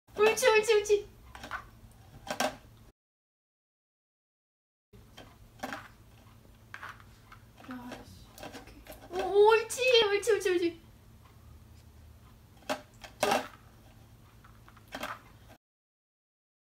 답답함주의! 새 장난감 살살 만지는 소심한 강아지 This puppy is very timid
*민도토리 / TORI (MINDOTORI)
* 2015년 7월 7일생 , 여 / Tori was born on July 7, 2015 ,  a girl.
* 토리인스타 / Tori's instagram